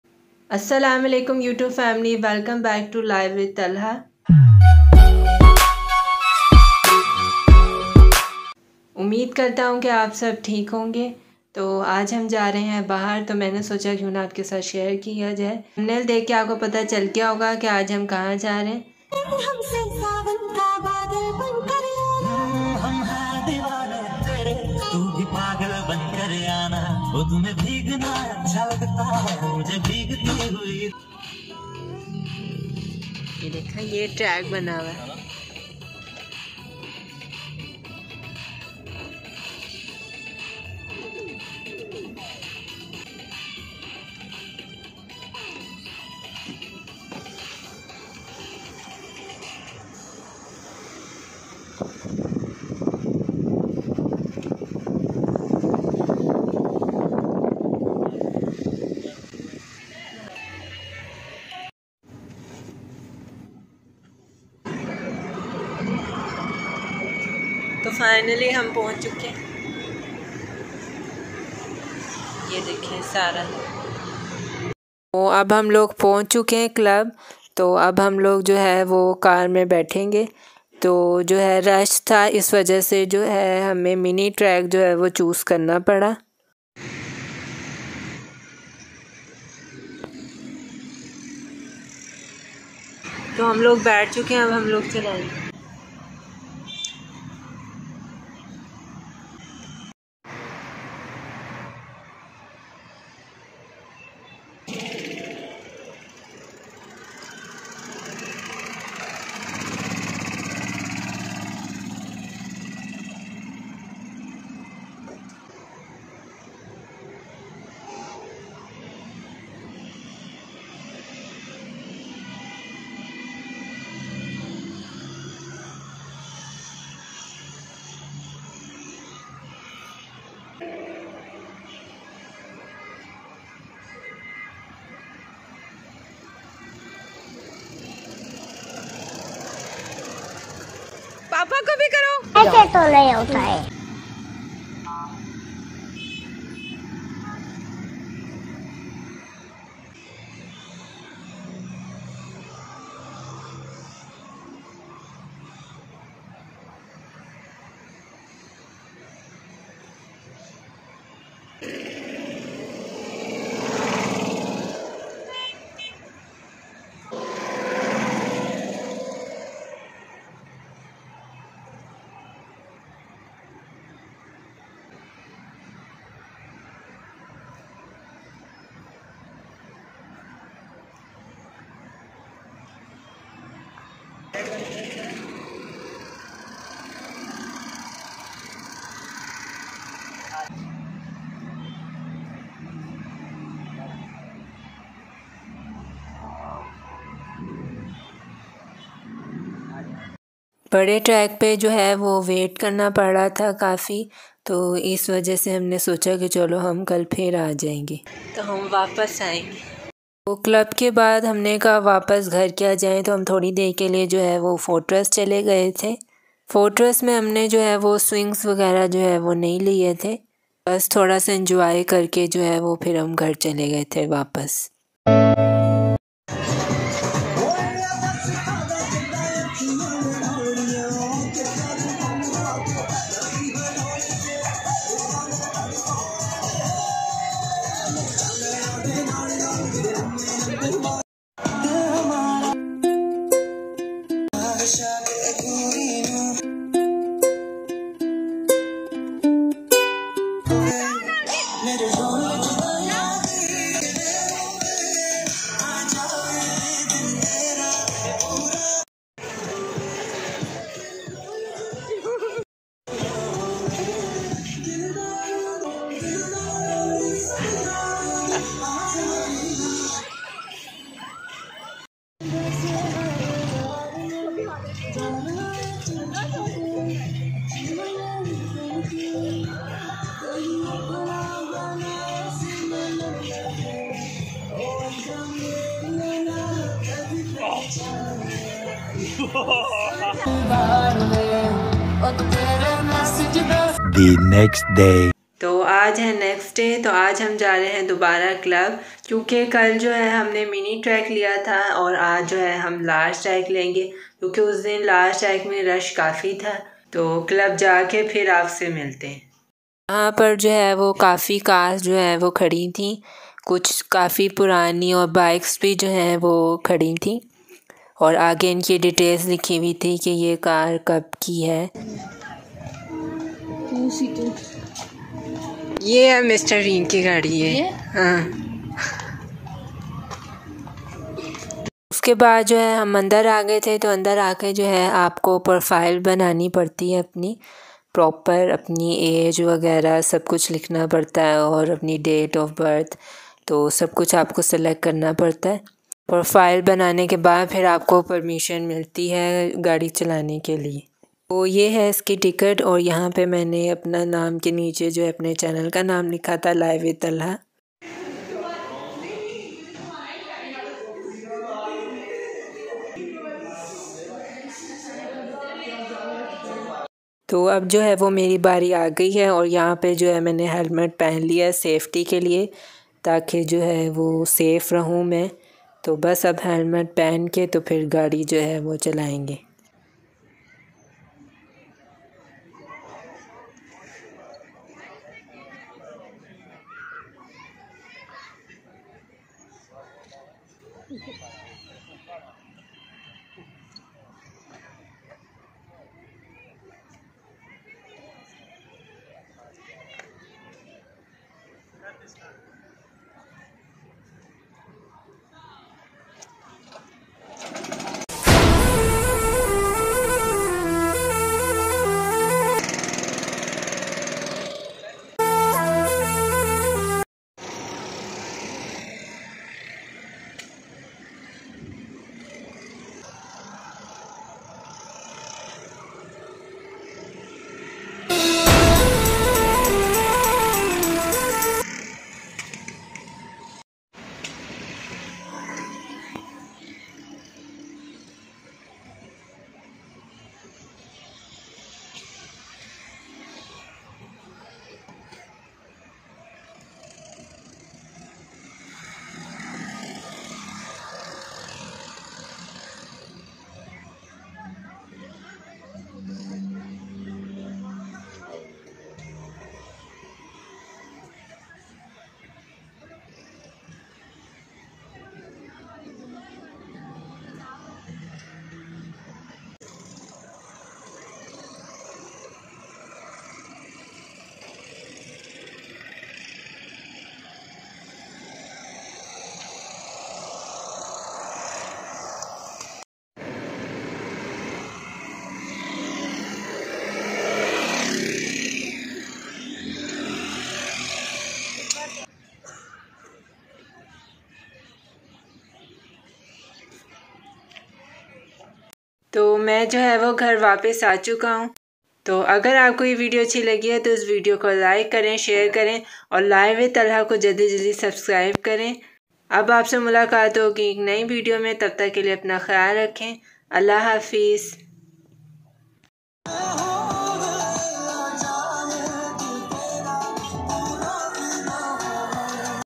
2F 2F karting Club Visit 🏁🏎️| Camel Ride 🐪 Life With Talha ❤️
2F 2F karting Club Visit 🏁🏎️ and Fortress Camel Ride 🐪

Life With Talha ❤️